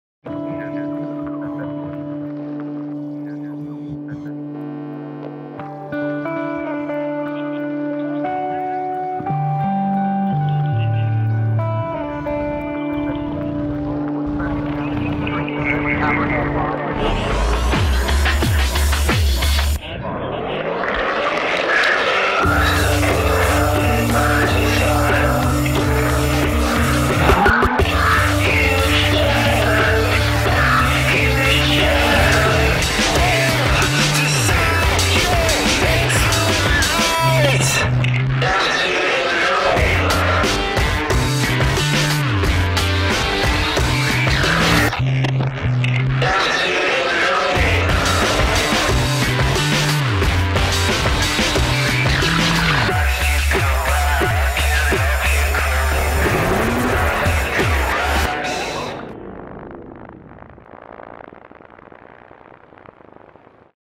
RIP CURL CREW
Video by Rip Curl
Rip Curl Crew (Mick Fanning, Steph Gilmore, Matt Wilkinson, Owen Wright) At Sunway Lagoon in Malaysia